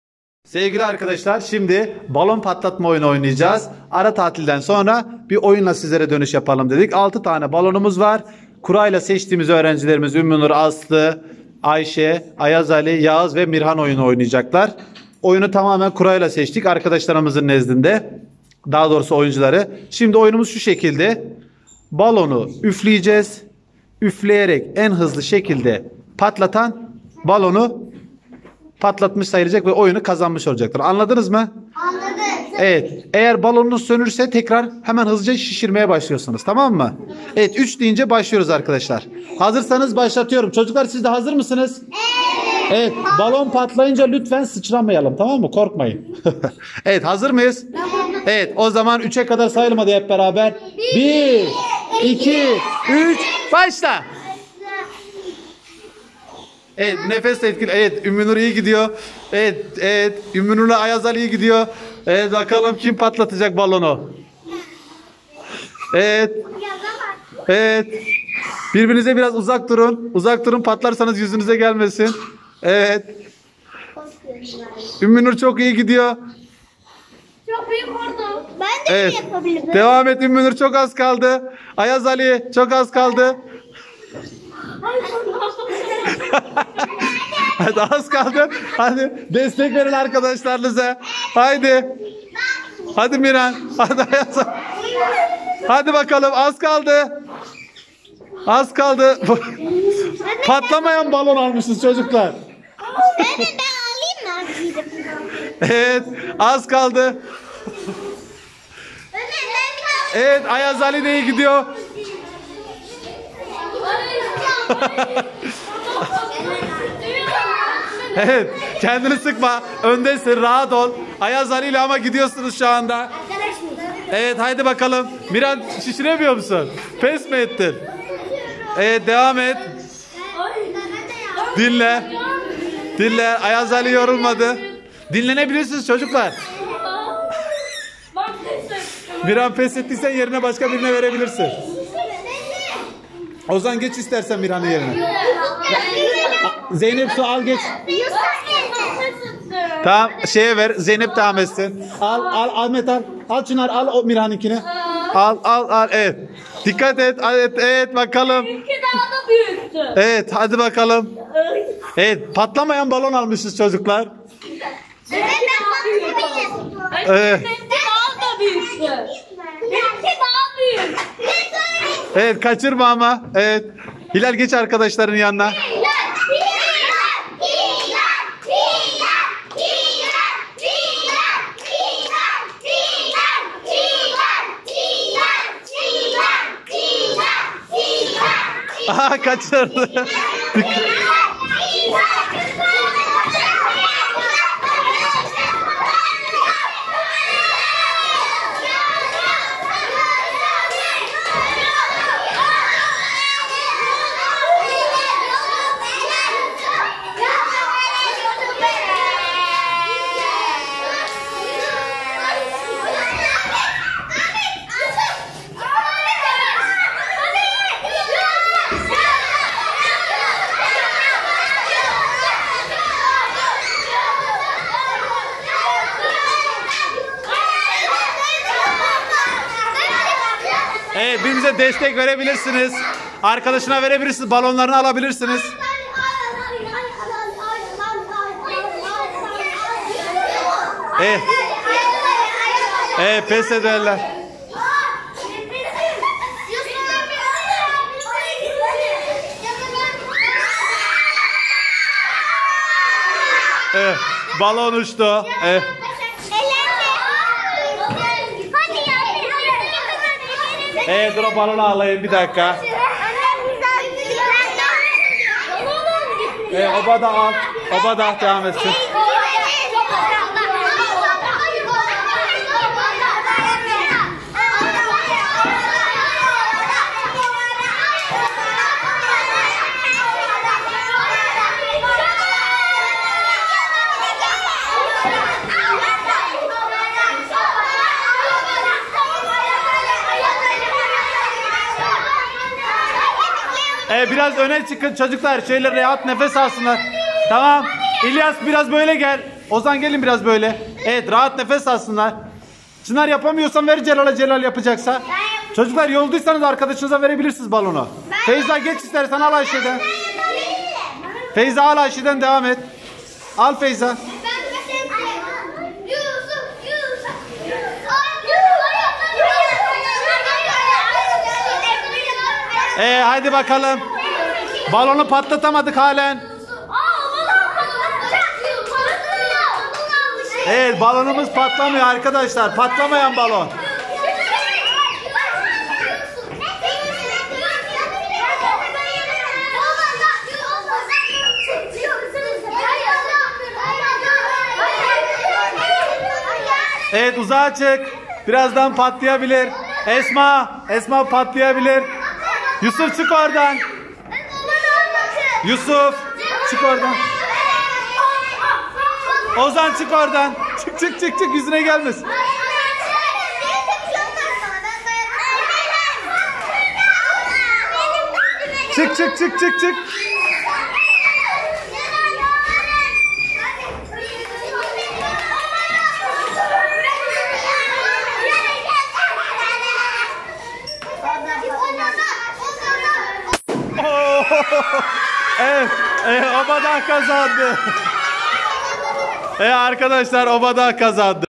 Şişirerek Balon Patlatma Oyunu
Ara tatilden sonra eğlenceli bir oyunla geri dönüş yaptık 🤗Balonlarımız büyük ve kaliteli olunca patlatmak biraz zor oldu 😁😁Ekip işi ile halloldu 😀Çocukların keyifli hareketleri mutluluğumuzu arttırdı. Nefes egzersizlerini ara ara yaptırmamız gerekir arkadaşlar 😉